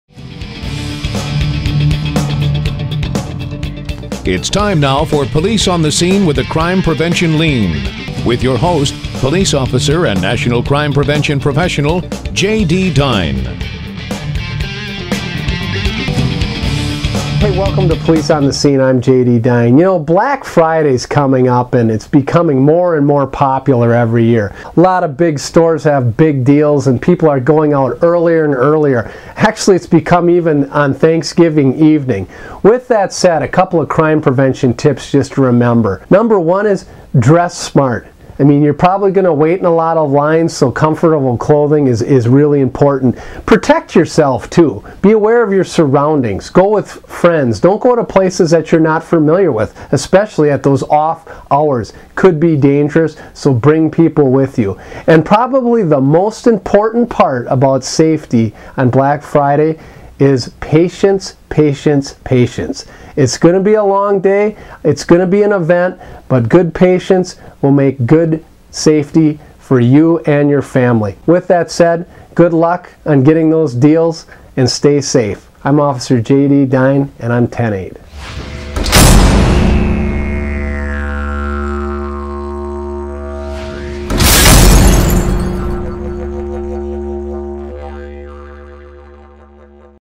Christmas Shopping Safety
Police on the Scene with a Crime Prevention Lean

Episode 277
J.D. Dhein lays it out how to be safe this holiday season. Preparation is key and with these three helpful tips, you will be prepared for whatever the holiday season has IN STORE for you. (See what I did there? Wasn't that pun?) Merry Christmas!